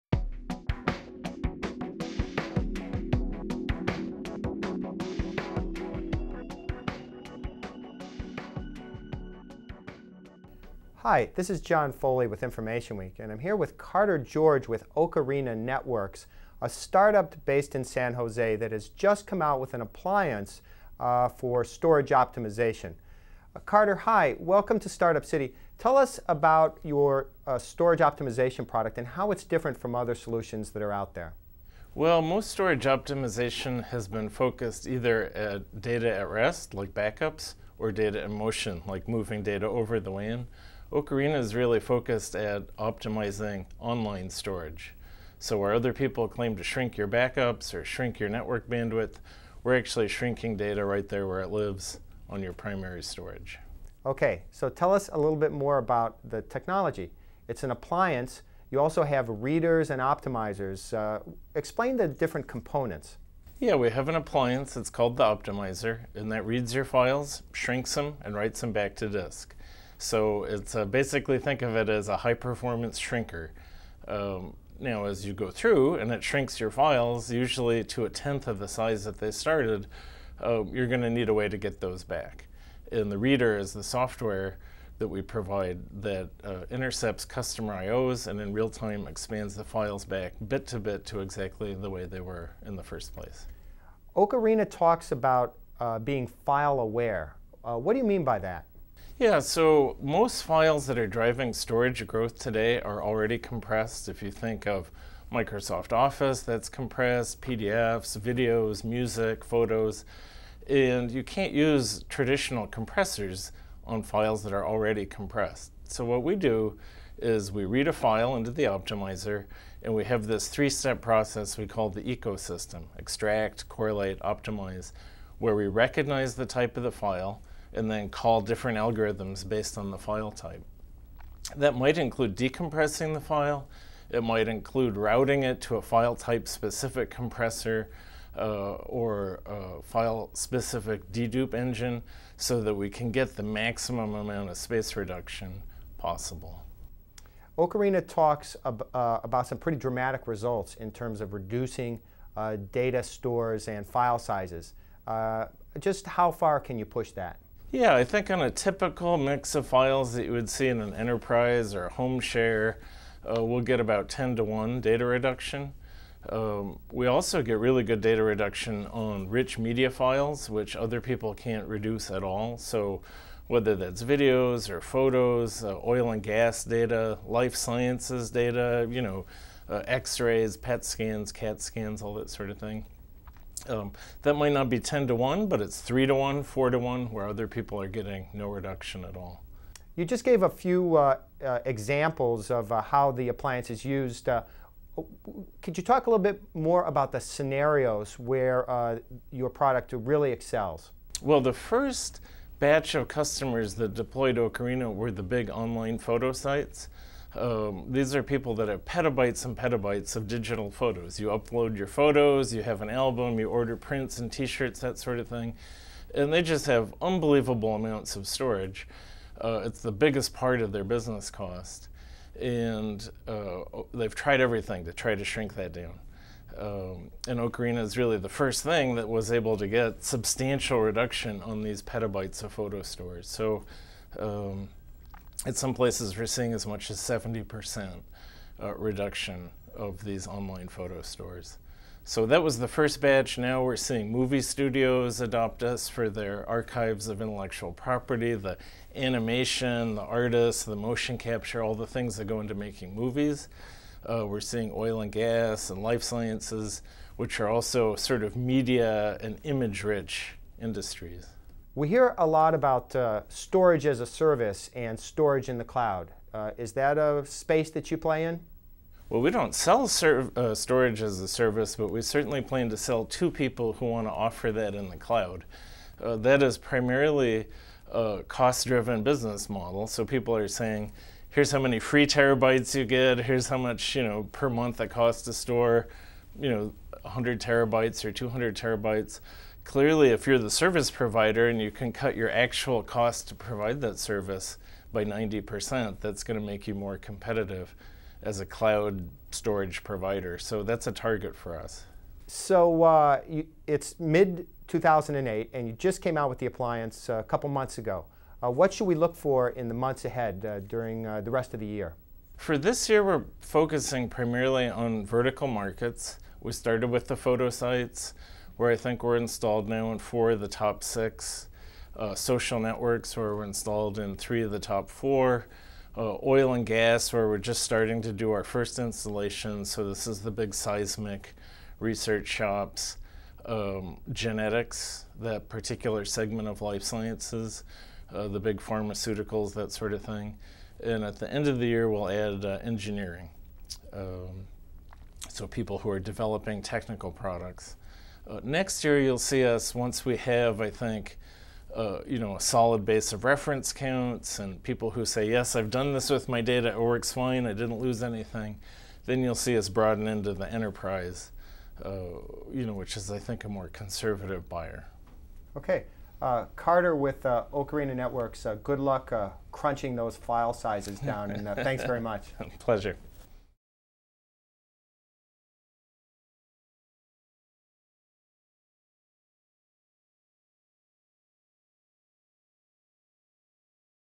Startup - Ocarina Networks
InformationWeek's John Foley speaks with Carter George, the Vice President of Products for Ocarina Networks. Unlike many storage optimization solutions, Ocarina Networks offers storage optimization that is focused on online storage.